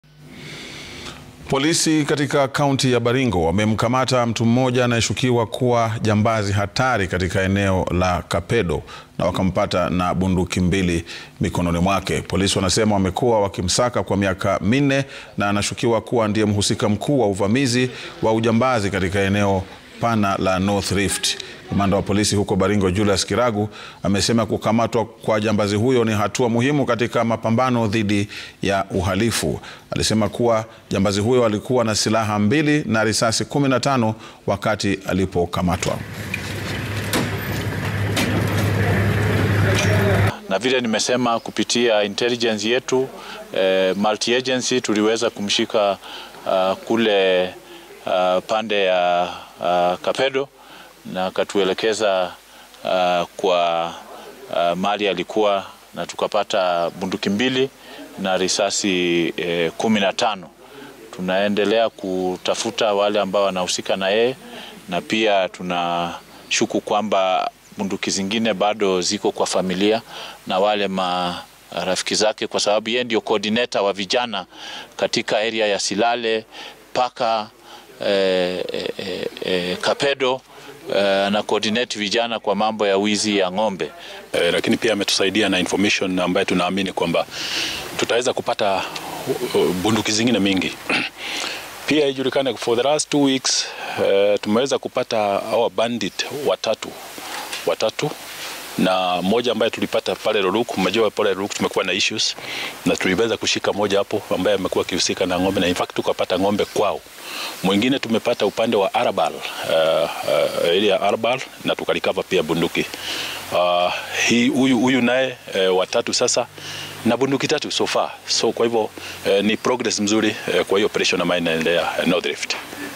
Polisi Baringo wamkamata mshukiwa wa ujangili
Polisi katika Kaunti ya Baringo wamemkamata Lonyangapat Amerinyang', jambazi anayeshukiwa kuwa hatari, katika eneo la Kapedo Akoret, Tiaty na bunduki mbili mikononi mwake.